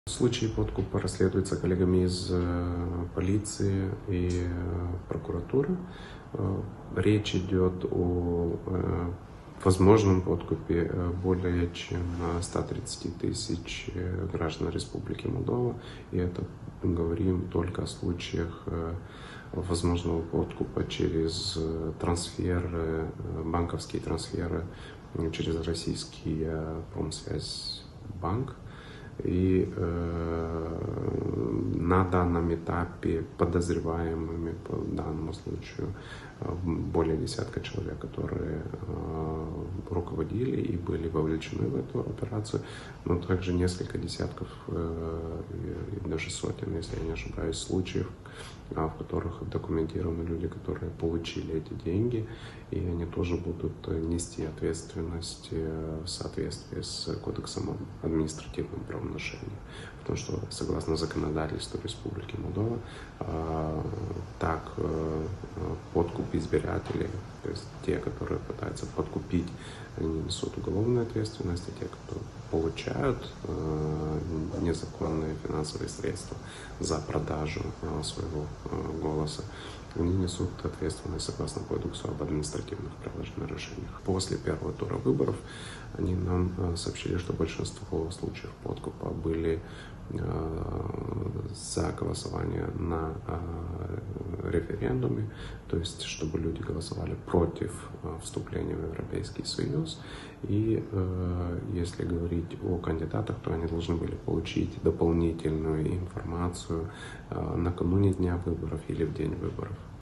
Массовый ПОДКУП избирателей во время первого тура выборов в Молдове: дело рук Кремля
На сегодняшний день полиция и прокуратура расследуют случаи подкупа избирателей во время первого тура президентских выборов. Речь идет о возможном подкупе более чем 130 тысяч граждан Республики Молдова, а именно через банковский трансфер российского "Промсвязьбанка". Об этом в эксклюзивном комментарии корреспонденту телеканала FREEДОМ Ларисе Петрук рассказал заместитель председателя Центральной избирательной комиссии Молдовы Павел Постика.